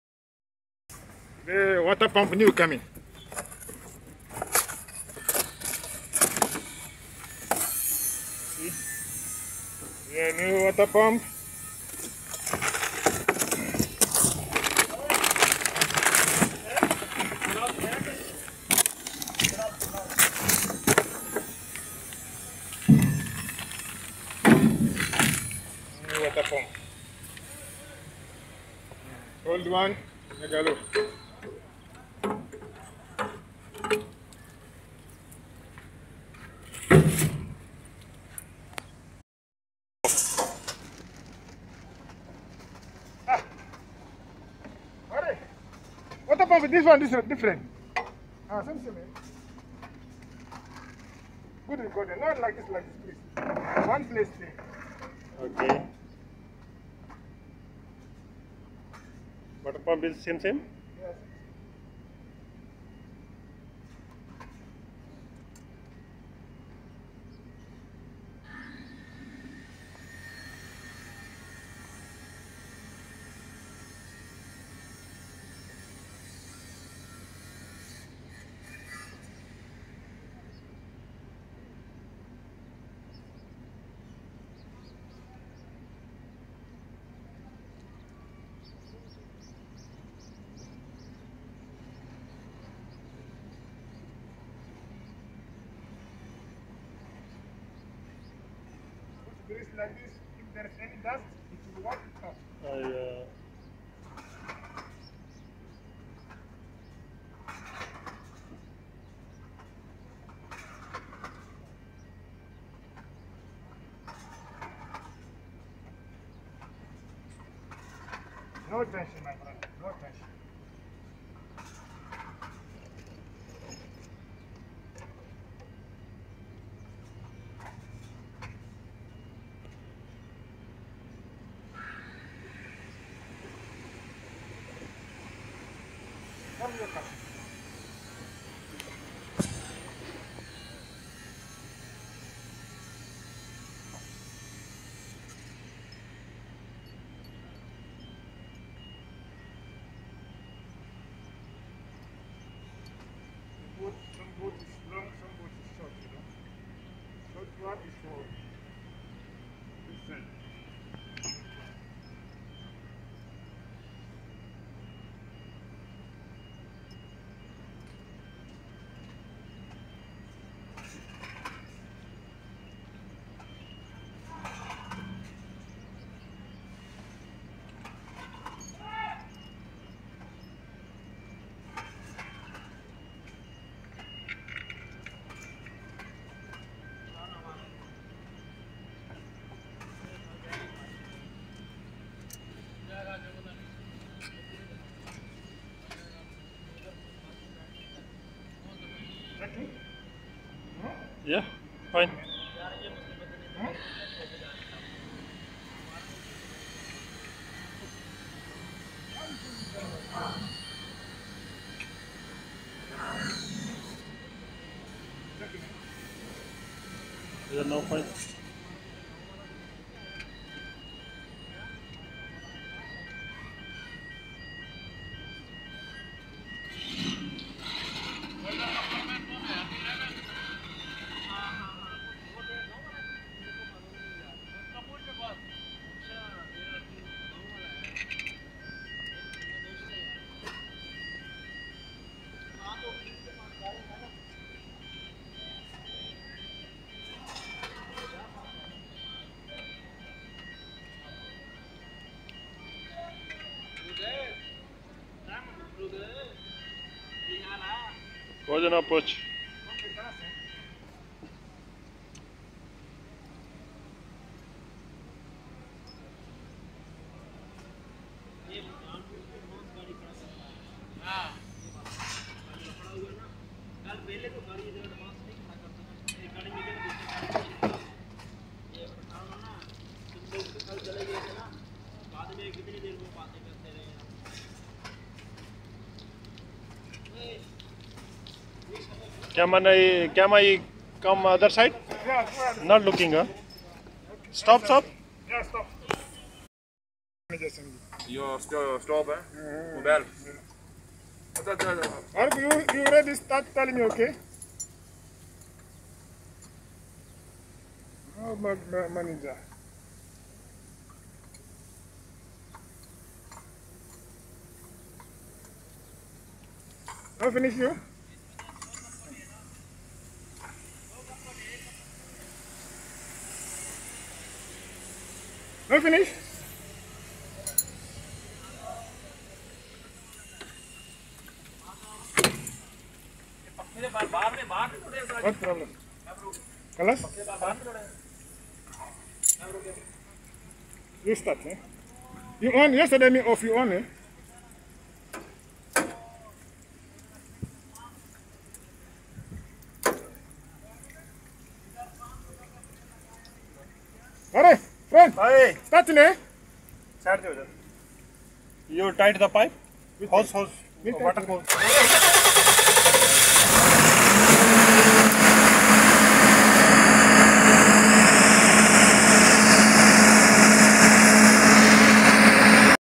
The Complete Guide to Assembling the Generator 150 KVA Water Pump
Overheat generator 150 KVA leaking water pump is the cost. A GUID to Assemble the Generator 150 KVA Water Pump. water pump diesel generator